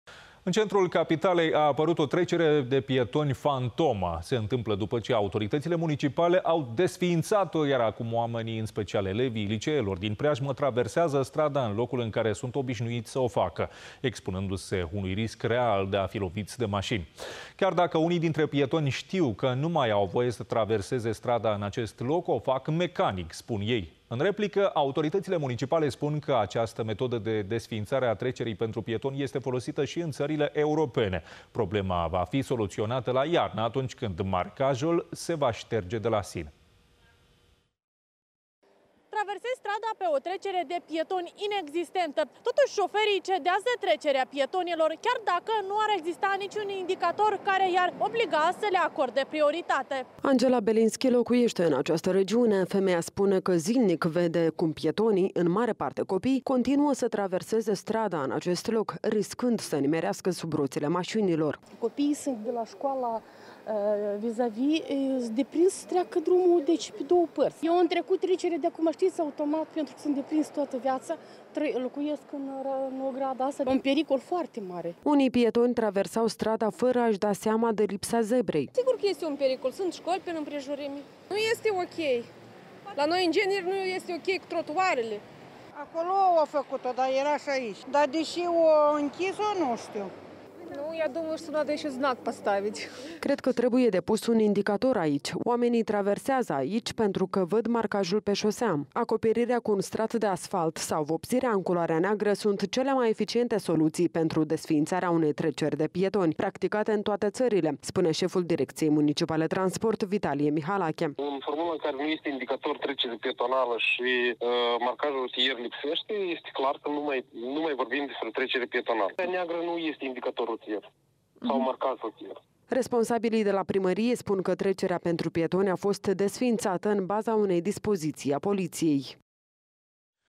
Trecere de pietoni „fantomă” în centrul capitalei
În centrul capitalei a apărut o trecere de pietoni fantomă. Se întâmplă după ce autorităţile municipale au desfiinţat-o, iar acum oamenii, în special elevii liceelor din preajmă traversează strada în locul în care sunt obişnuiţi să o facă, expunându-se unui risc real de a fi loviţi de maşini.